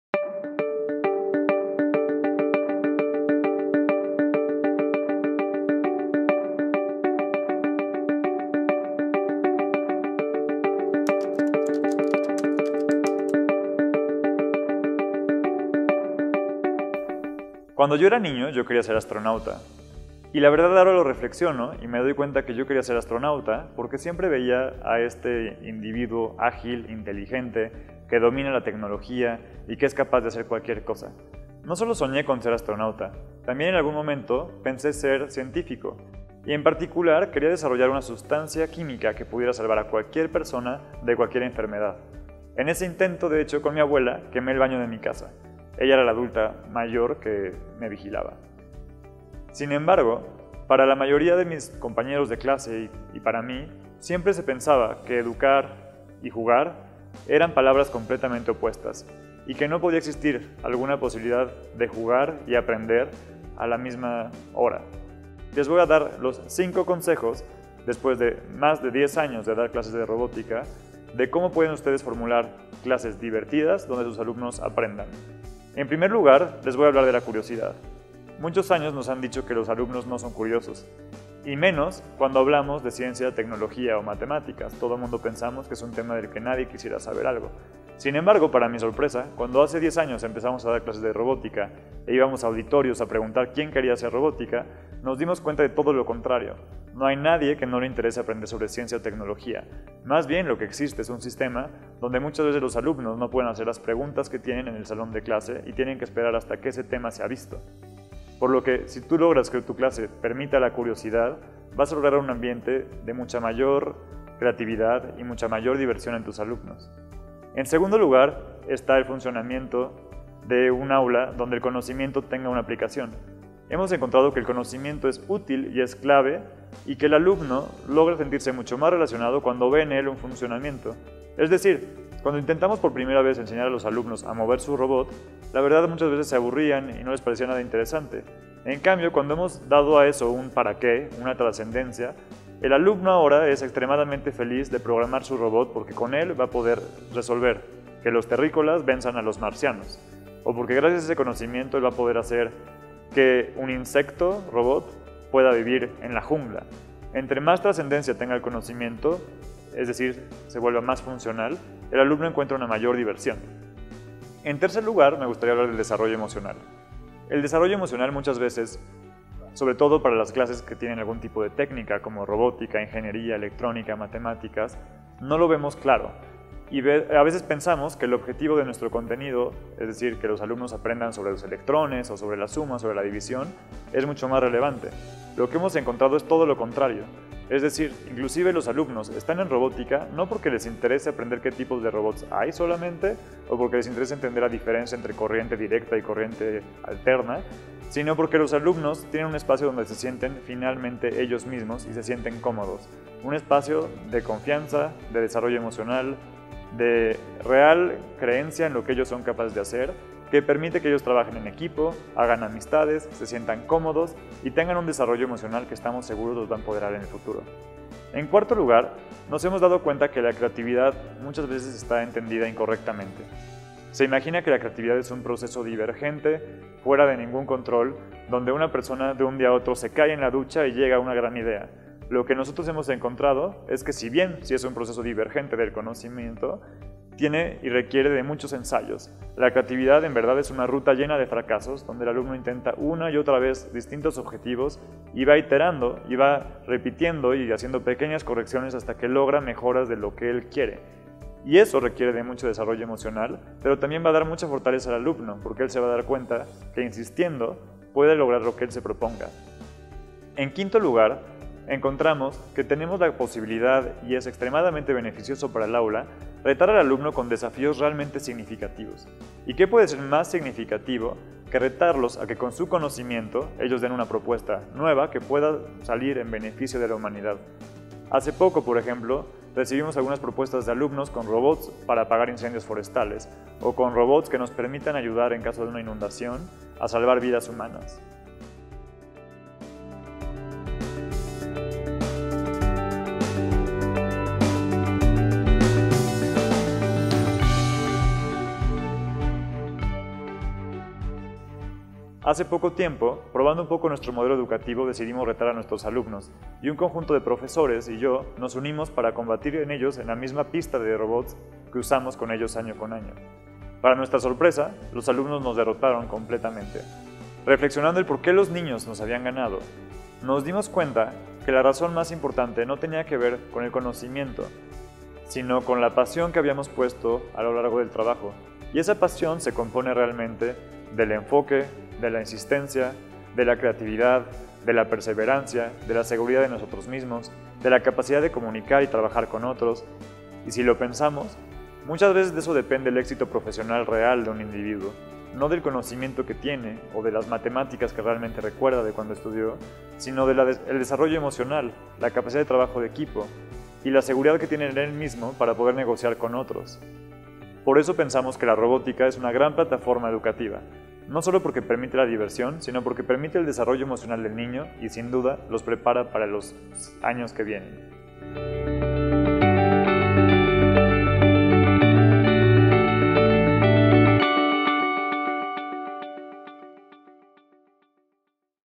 La robótica como plataforma educativa | Compartir en Familia
Roberto Saint Martin (Ingeniero)

La robótica educativa es, hoy en día, una de las herramientas imprescindibles para desarrollar habilidades y capacidades que ayudarán a los alumnos en su proceso formativo.

¡Apúntate a nuestra Newsletter mensual! Y sé el primero en conocer nuestras novedades.